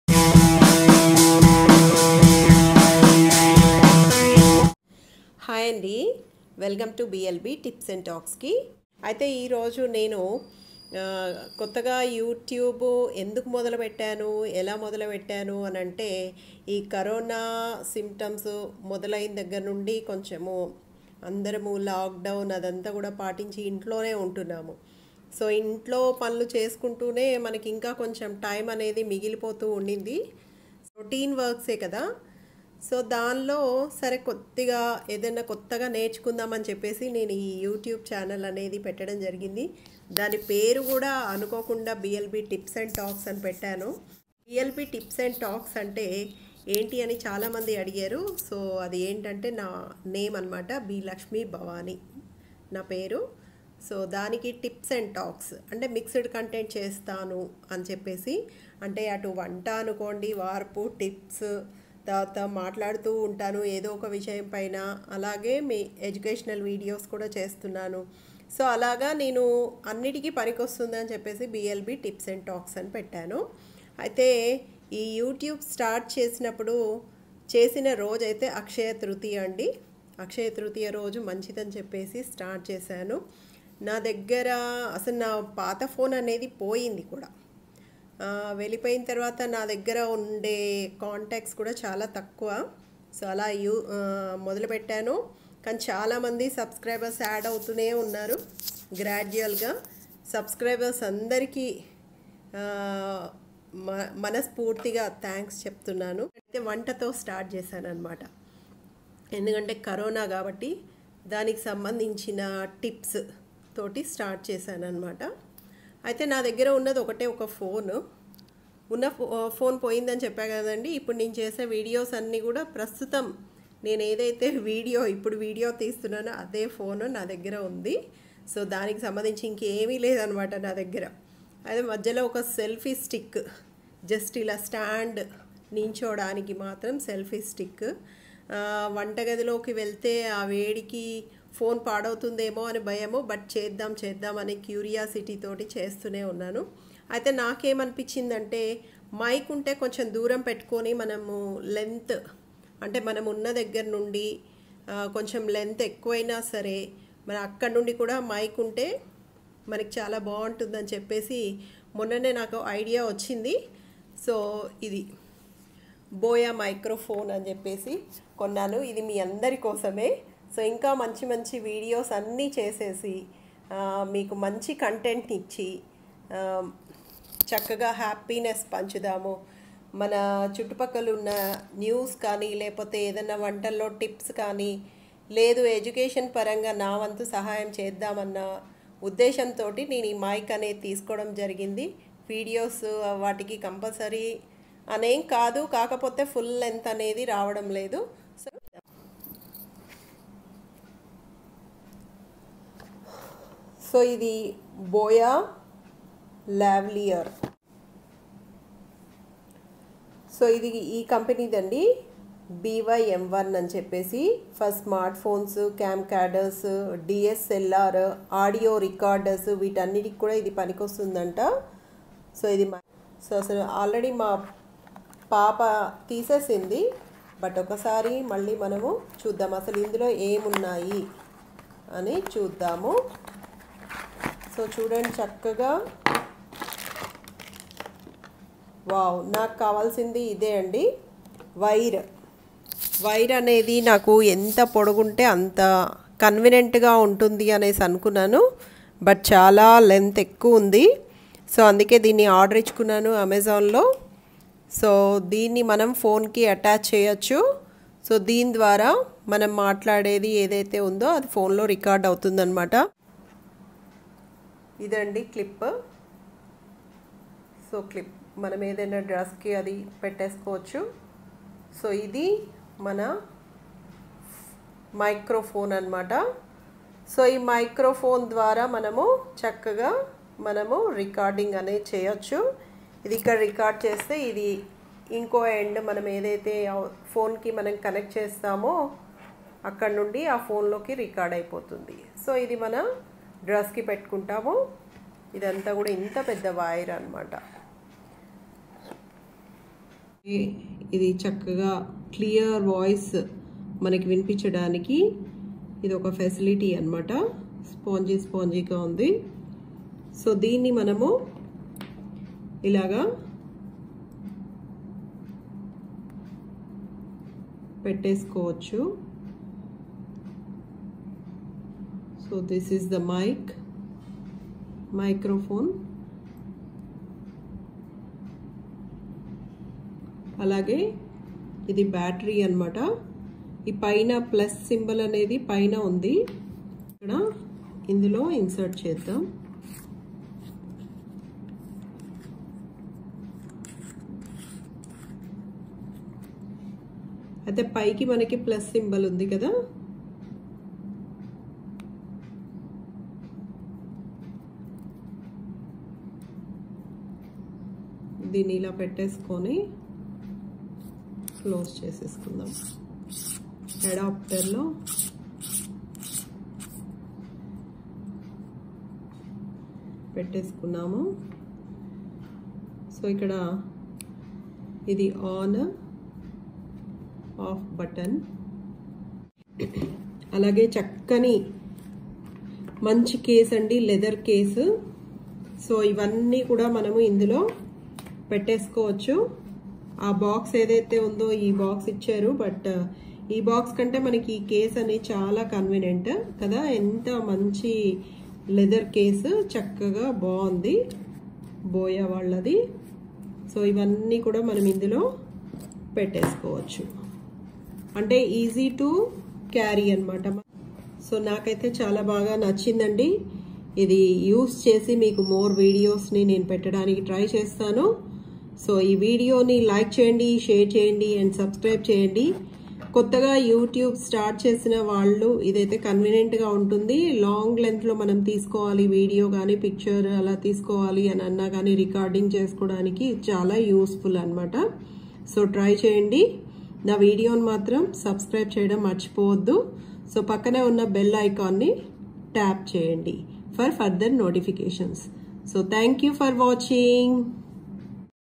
BOYA M1,Lavalier microphone @BLB tips and talks
BOYA BY- M1,Lavalier microphone,micro-cravate, for smartphones, Camcorders, DSLR Audio Recorders, PC etc.  Superb sound for presentations and video recording.